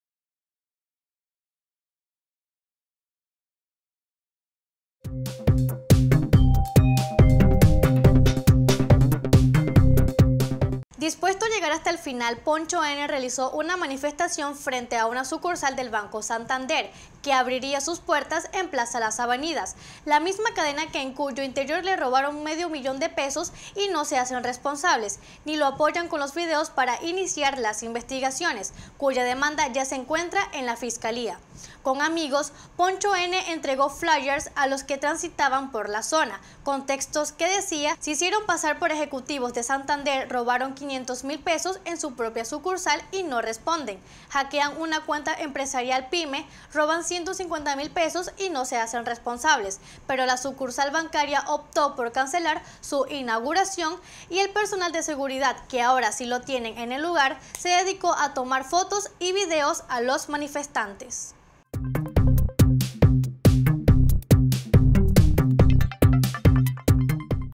PONCHO “N” PROTESTA EN SUCURSAL DE SANTADER Y EXIGE RESPONSABILIDAD BANCARIA 2
Entérate de las noticias “EN CORTO”…PONCHO “N” PROTESTA EN SUCURSAL DE SANTADER Y EXIGE RESPONSABILIDAD BANCARIA...